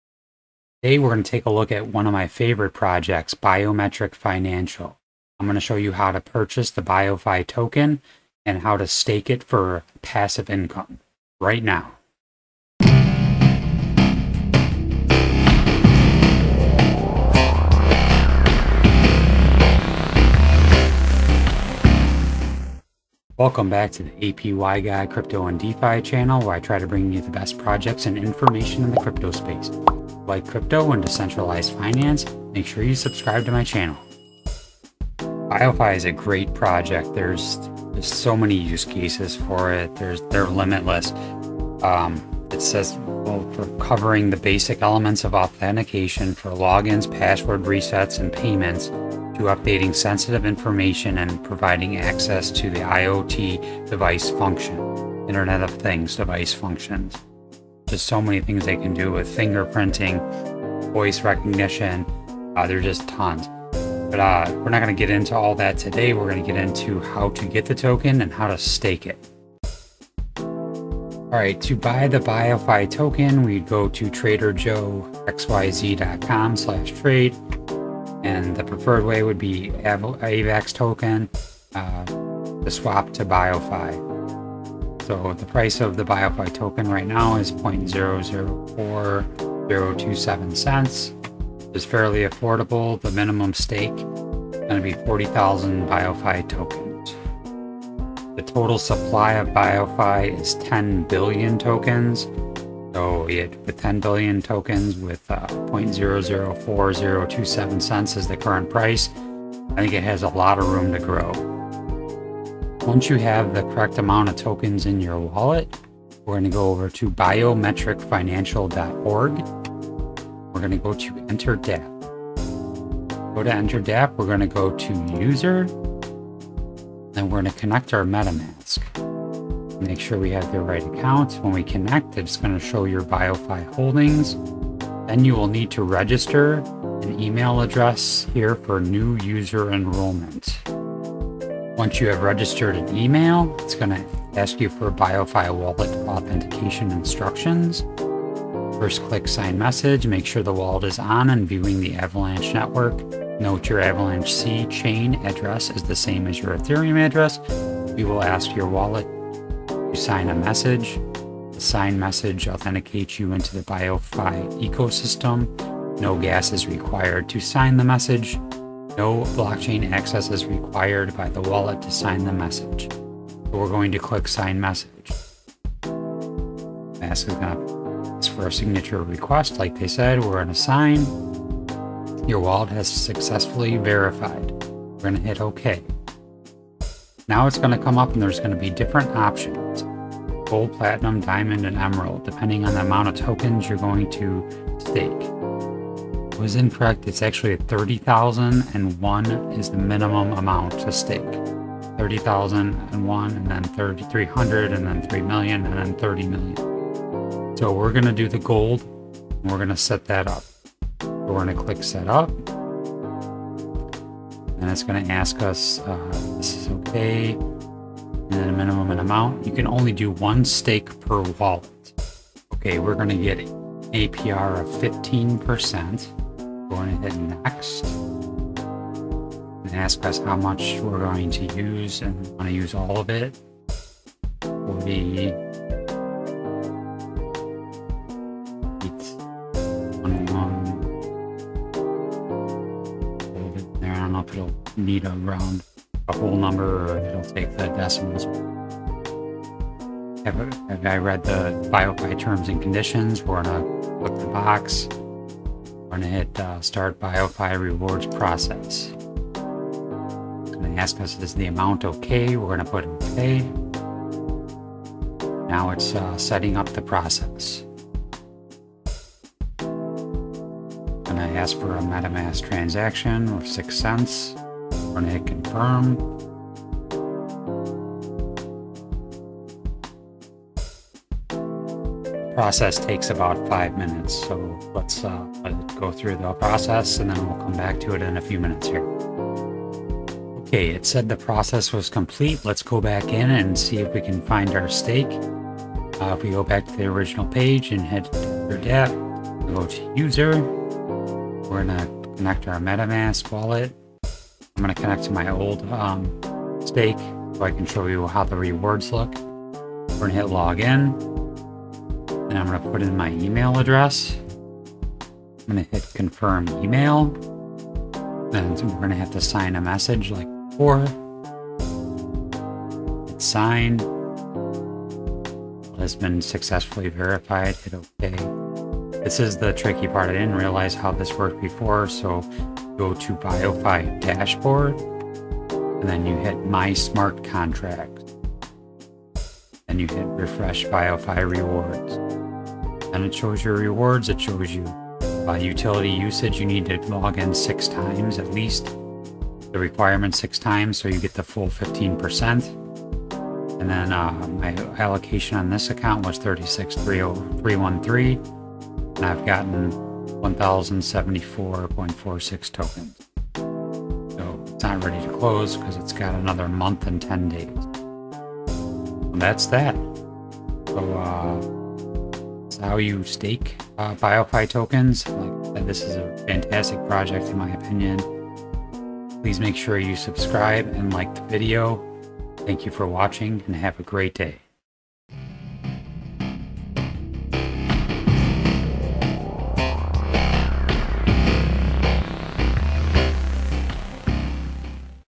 How To Stake $Biofi Biometric Financial For Passive Income
Biometric Finance ($BioFi) is a utility token that serves as the glue holding together a
secure set of solutions developed by Finnovant, Inc., and Finnovant key providers. The BioFi
ecosystem leverages biometric security to provide decentralized finance (DeFi) products and
services riding on the blockchain. Finnovant’s core biometric solutions rely on our Say-Tec voice
and facial authentication product line, now being used in the ecosystem globally.

2022,crypto live stream,crypto live trading,crypto trading live,live crypto
trading,10x crypto